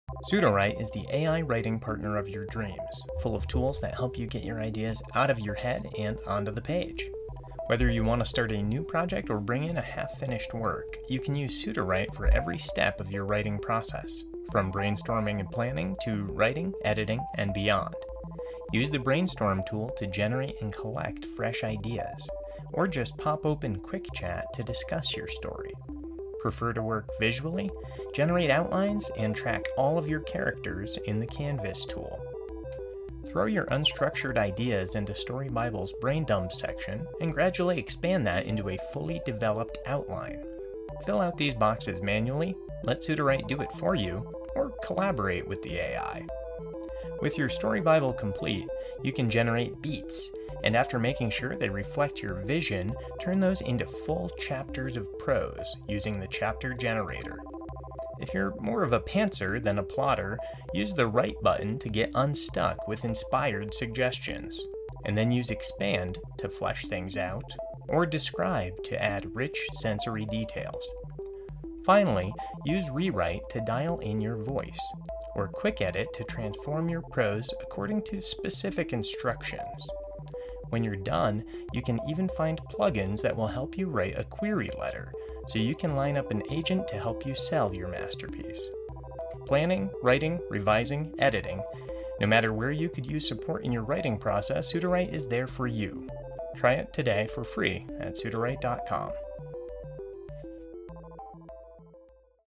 What does Sudowrite do? AI writing features explained
Sudowrite is a powerful and flexible AI writing toolkit made just for authors. But what does it actually *do*? This is an introduction to the wide range of features available on Sudowrite—a brief showcase of exactly how you can use Sudowrite to plan, edit, write, and organize your work. ✨

New to AI writing? Give it a spin for free today